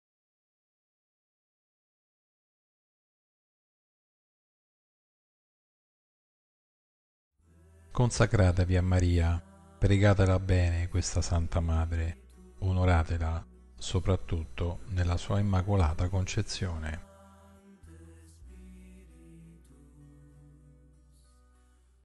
Le Parole dei Santi per oggi, 15 Ottobre 2023 - San Giovanni Maria Vianney
"Consacratevi a Maria. Pregatela bene questa Santa Madre, onoratela soprattutto nella sua Immacolata Concezione".

"Consecrate yourselves to Mary. Pray well to this Holy Mother, honour her especially in her Immaculate Conception".

"Consacrez-vous à Marie. Priez bien cette Sainte Mère, honorez-la surtout dans son Immaculée Conception".

"Consagraos a María. Rezad bien a esta Santa Madre, honradla especialmente en su Inmaculada Concepción".

"Consagrai-vos a Maria. Rezai bem a esta Santa Mãe, honrai-a especialmente na sua Imaculada Conceição".